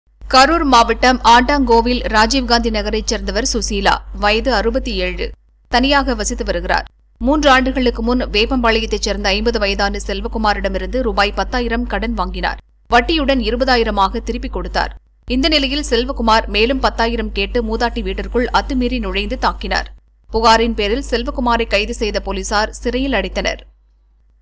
ரூ.10 ஆயிரத்துக்கு 20 ஆயிரம் வட்டி மூதாட்டியை தாக்கியவர் கைது!
ரூ.10 ஆயிரத்துக்கு
20 ஆயிரம் வட்டி
மூதாட்டியை
தாக்கியவர் கைது!கரூர்